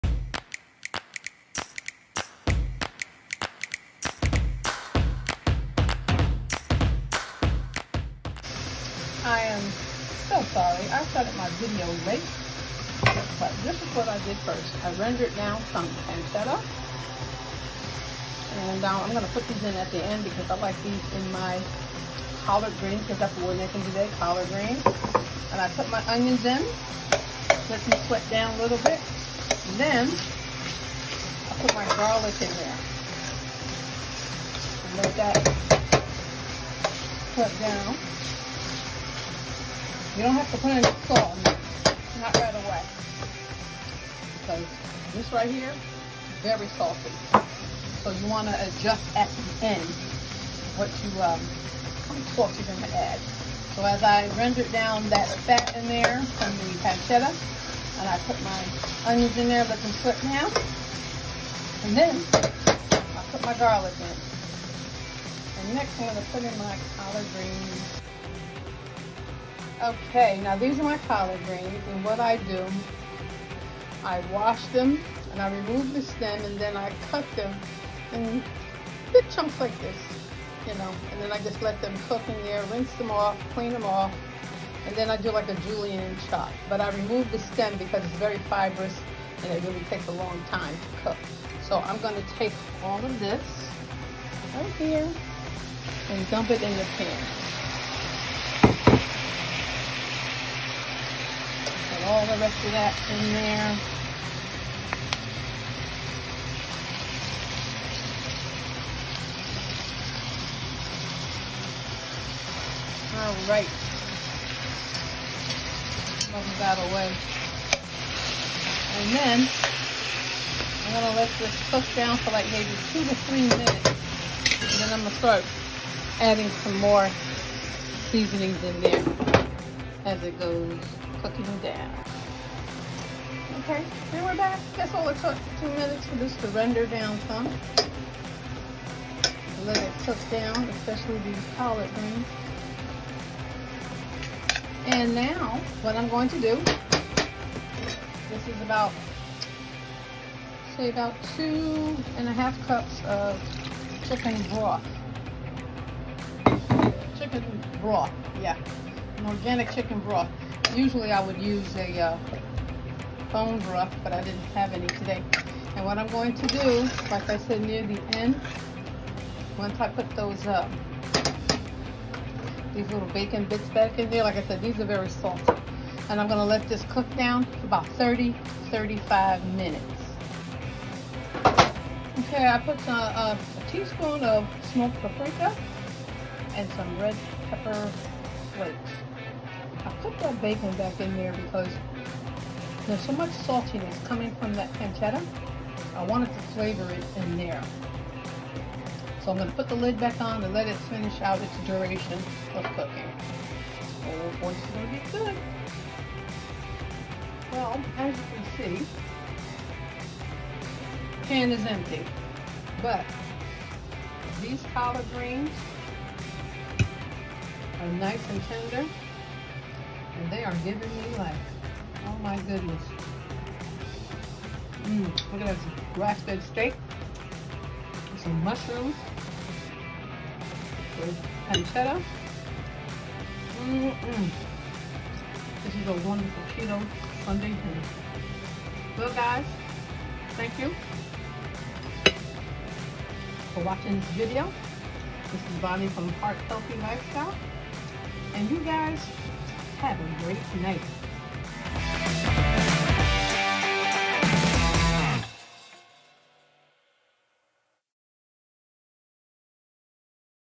Keto Collard Greens 🥬
1/2 cup Onions
2 cloves Garlic
2 Tblsp Evoo
1/4 cup of pancetta or bacon
3 cups of broth

Spices:
Smoked paprika
Red pepper flakes
Salt
Pepper

Heat oil in a large pan over medium-high heat. Add pancetta , and cook until crisp. Remove pancetta from pan, Add onion, and cook until tender, about 5 minutes. Add garlic, and cook until just fragrant. Add collard greens, and sauté until they start to wilt (2 minutes).

Pour in chicken broth, and season with salt, pepper, paprika and red pepper flakes. Reduce heat to low, cover, and simmer for 30-35 minutes, or until greens are tender.

Thank you for taking the time and watching my video😊
Don’t forget to give this video a 👍🏽